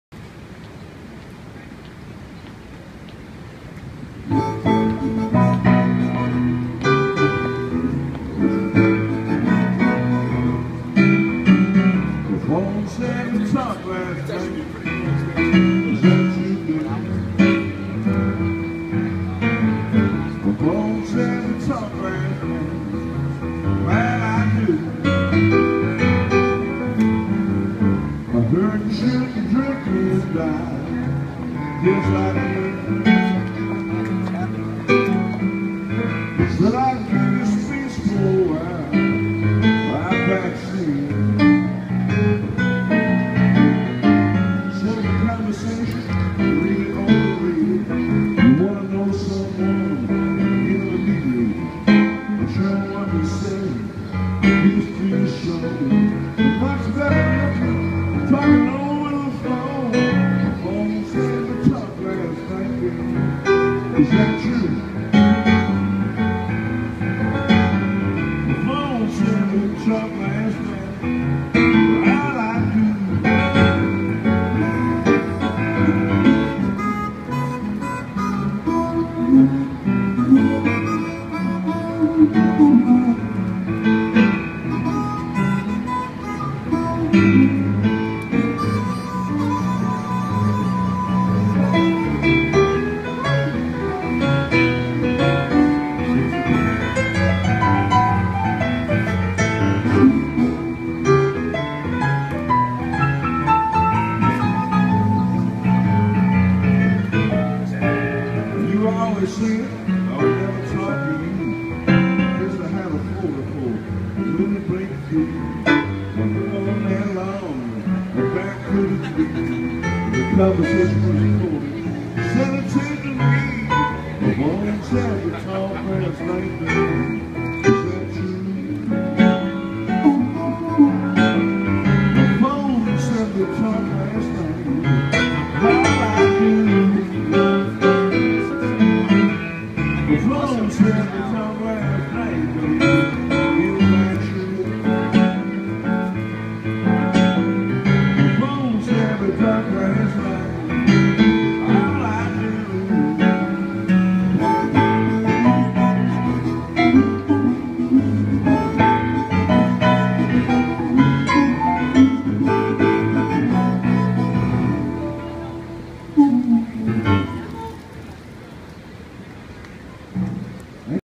Blues in the Park
Afternoon entertainment in Post Office Square park.  Thanks, Tim.